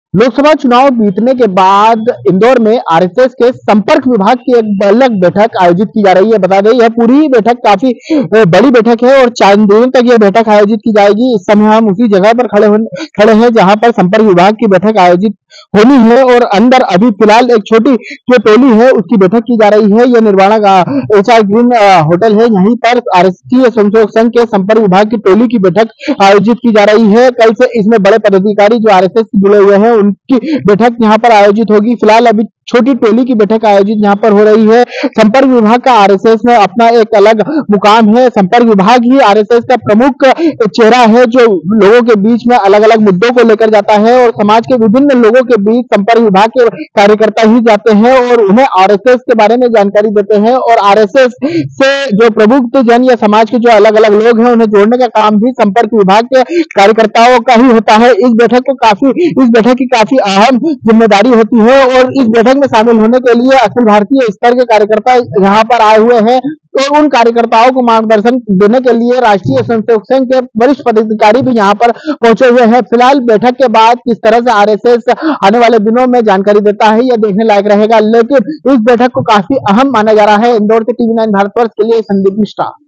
Indore में RSS की चार दिवसीय बैठक का आगाज, 150 से ज्यादा पदाधिकारी होंगे शामिल ।। MP News
राष्ट्रीय स्वयंसेवक संघ की चार दिवसीय संपर्क विभाग की बैठक इंदौर में आज से शुरू हो रही है इस बैठक में भगाने के लिए आरएसएस के संपर्क विभाग से संबंधित कई पदाधिकारी इंदौर पहुंच चुके हैं वहीं इस बैठक में विभिन्न मुद्दों को लेकर चर्चा होगी।

The four-day meeting of the Contact Department of Rashtriya Swayamsevak Sangh is starting from today in Indore. Many officials related to the Contact Department of RSS have reached Indore to attend this meeting. Various issues will be discussed in this meeting.